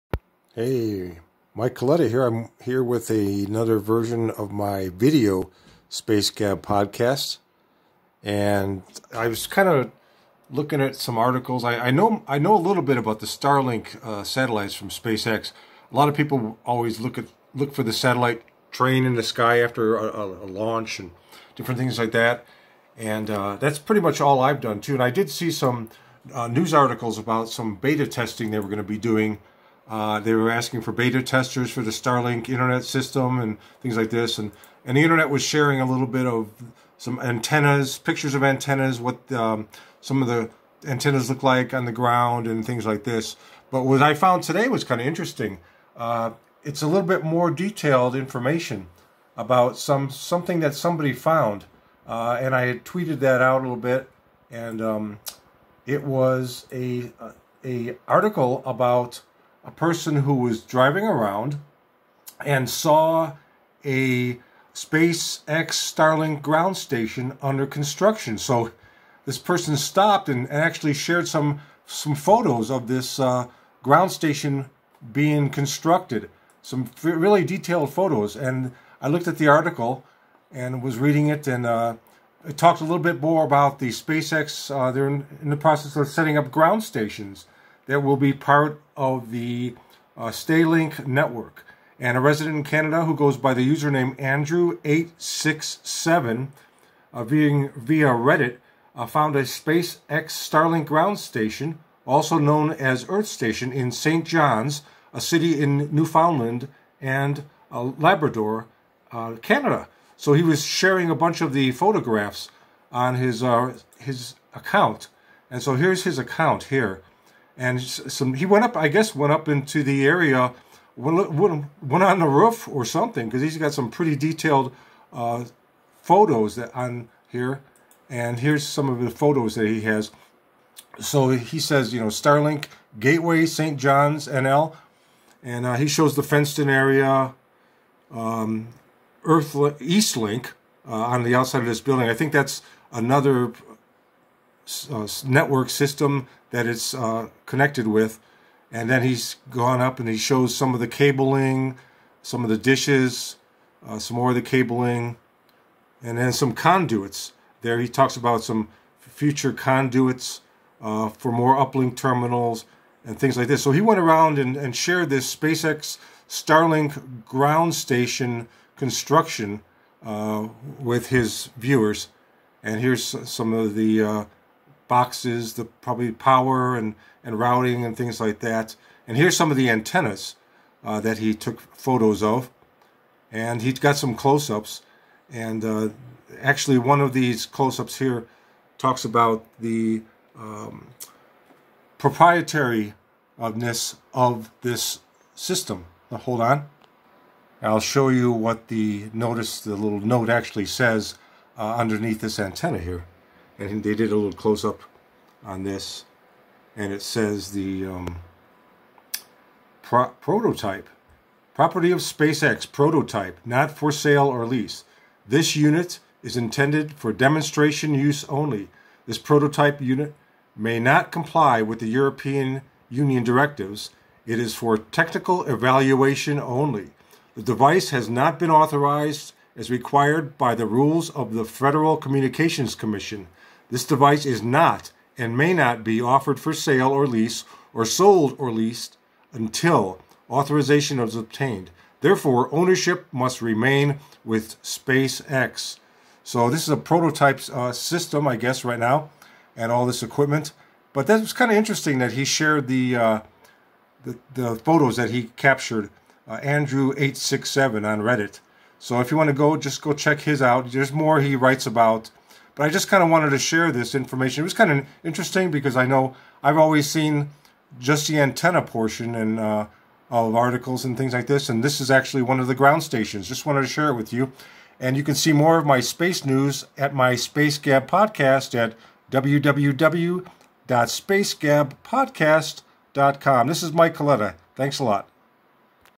SpaceX Starlink Prototype Ground Station photos exposed by Canadian resident
SpaceX Starlink Prototype Ground Station photos exposed by Canadian resident. SpaceX Starlink satellite ground Station photos.
For more space news visit my Space Gab Podcast